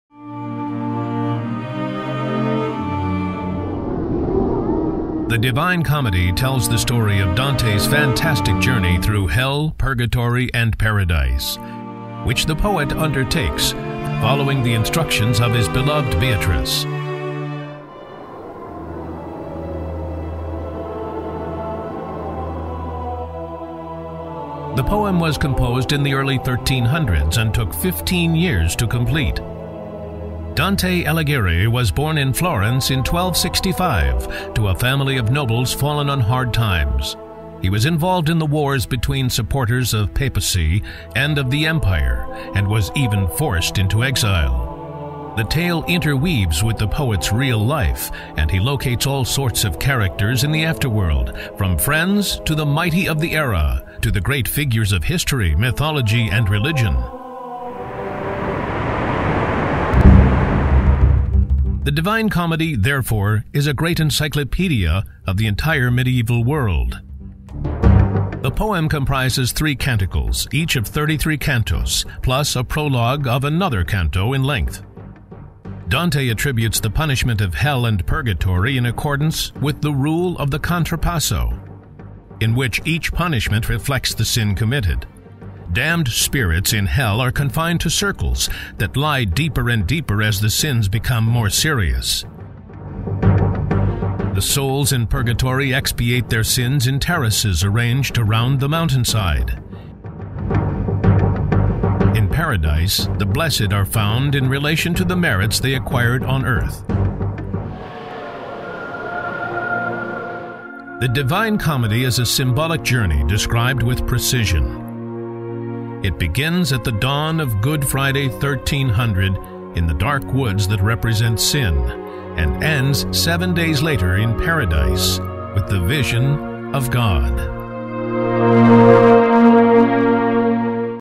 iDante. Introduction to Divine Comedy
In The Divine Comedy touch eBook, created by Gualtiero and Roberto Carraro with Fulvio Massini, a 3D model helps to understand and explore the three realms of the Afterworld. The touch eBook also contains a series of 360° panoramic reconstructions.
3D technology enables a very effective portrayal of  Hell, Purgatory and the heavens of Paradise.